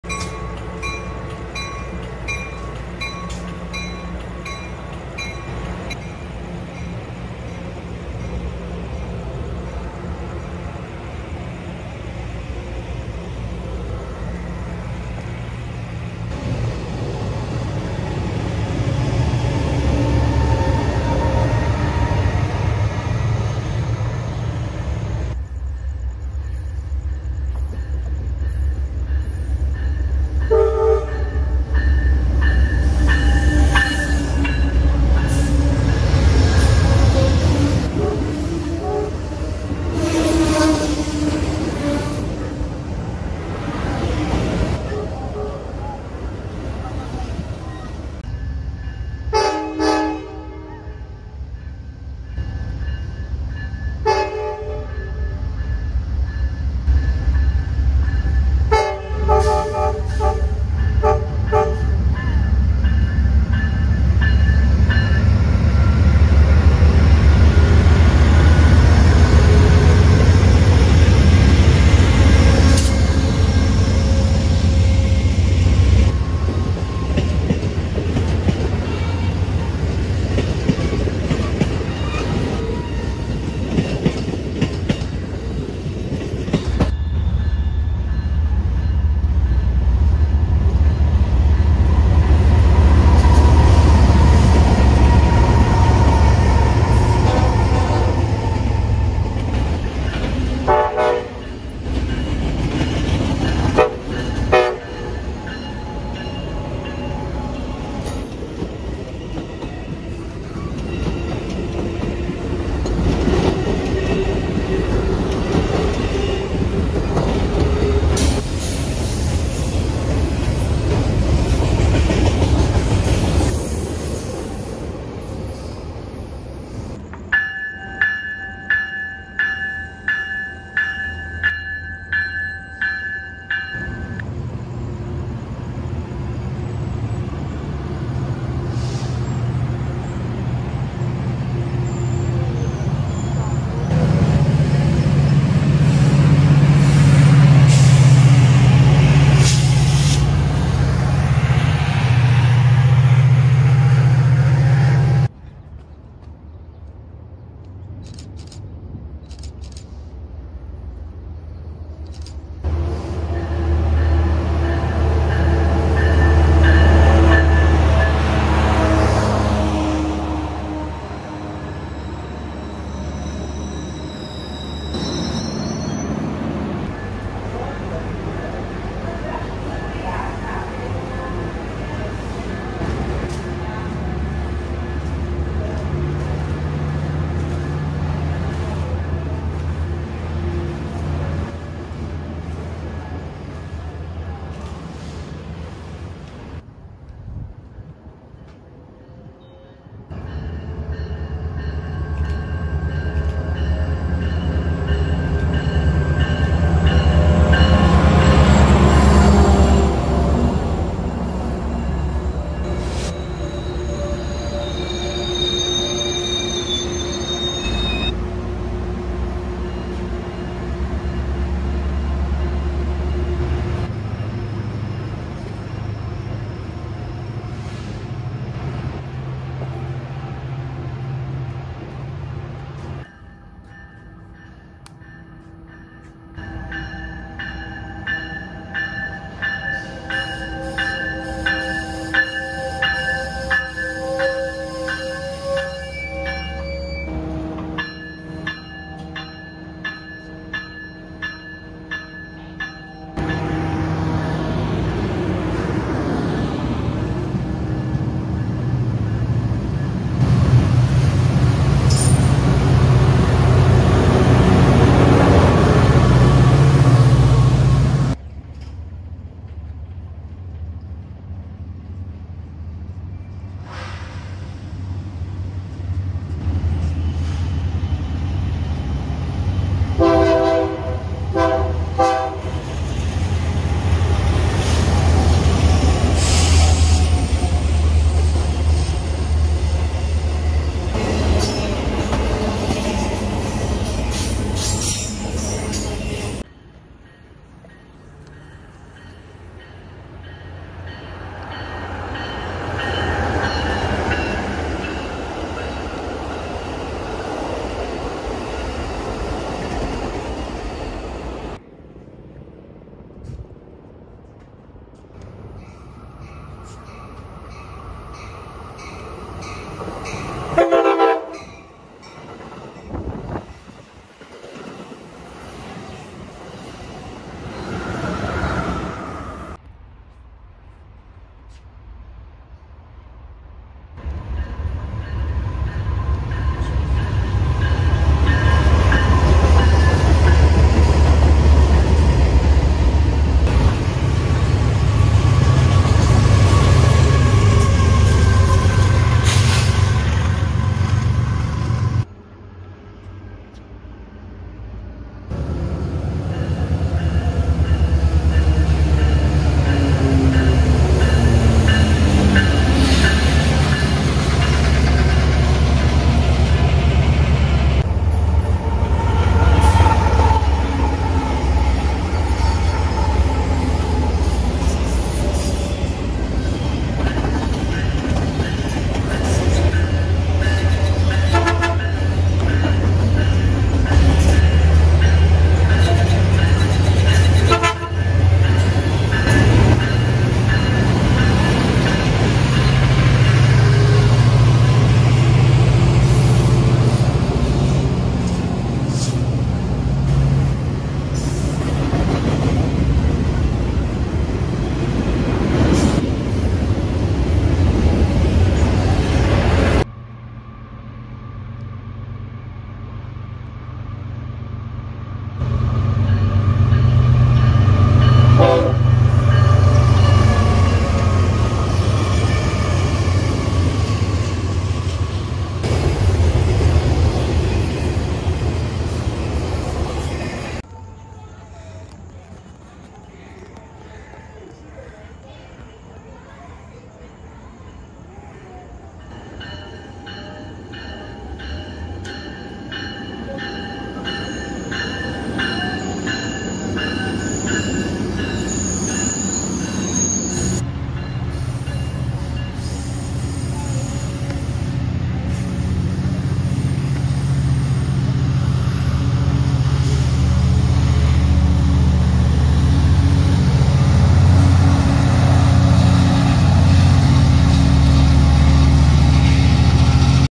Nighttime & evening trains in Northern Orange County! 2 Stations! Hornshows, KCS, and more!
Hey everyone! Today I decided to work on a train video after realizing how much clips I had where I filmed in the evening or at night! Some clips I filmed with @NotARailfan . Enjoy!

0:20 - 0:24: Amtrak 785
0:24 - 1:26: Amtrak 4
1:26 - 2:06: Amtrak 784 and BNSF Port Stack
2:07 - 2:34: Metrolink 667
2:35 - 2:37: Buena Park
2:38 - 3:15: Metrolink 710
3:16 - 3:52: Metrolink 606
3:53 - 4:28: Metrolink 689
4:29 - 4:56: BNSF Q Intermodal
4:57 - 5:11: Amtrak 784
5:11 - 5:29: Amtrak 785
5:30 - 5:51: BNSF Port Stack
5:52 - 6:49: Metrolink 608
6:50 - 7:09: BNSF Intermodal
7:10 - 7:14: The next day at Fullerton
7:15: Metrolink 667